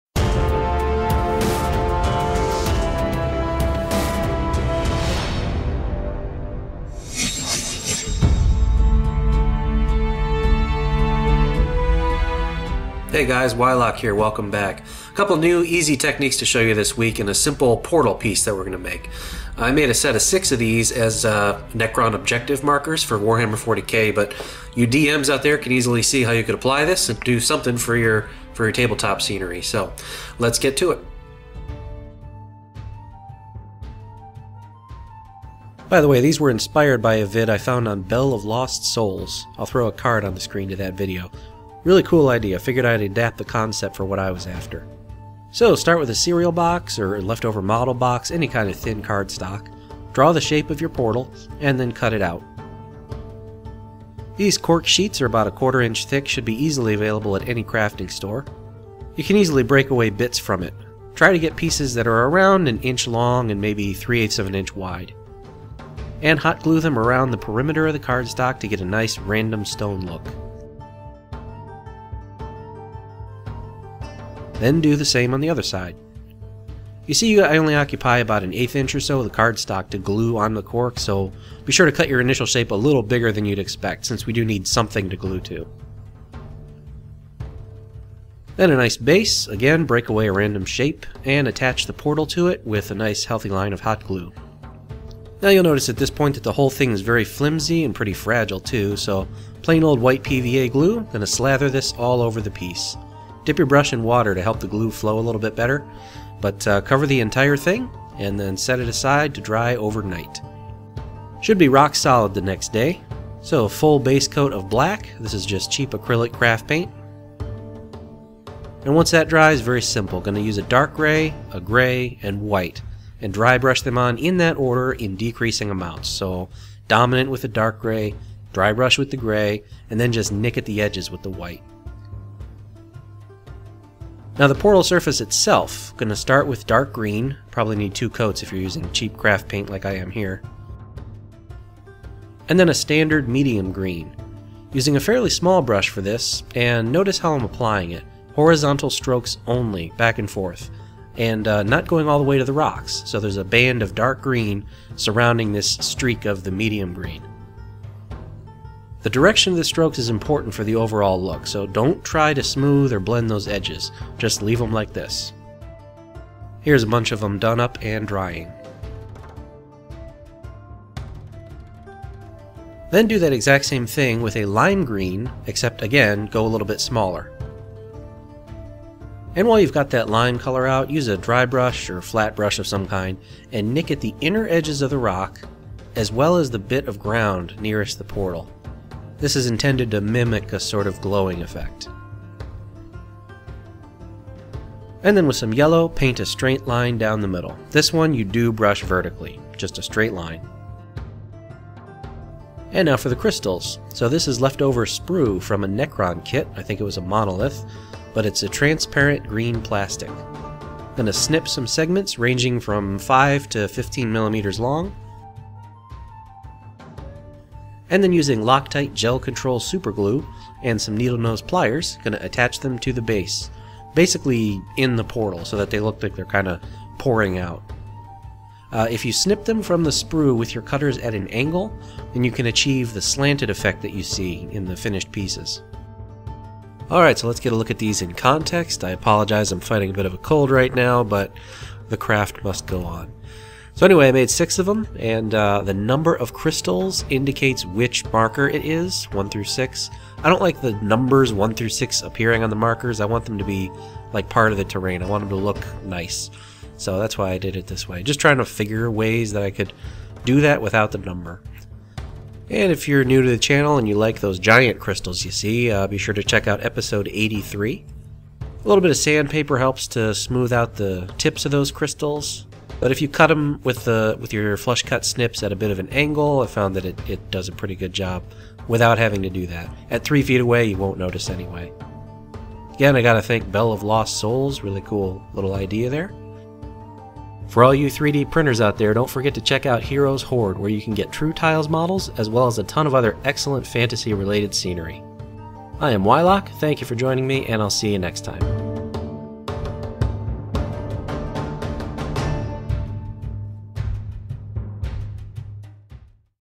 How to Make Necron Portal Objective Markers for Warhammer 40k
How to craft Necron portal objective markers for Warhammer 40k.

D&D 5e Modules

The home of TrueTiles and other excellent 3D-printable models.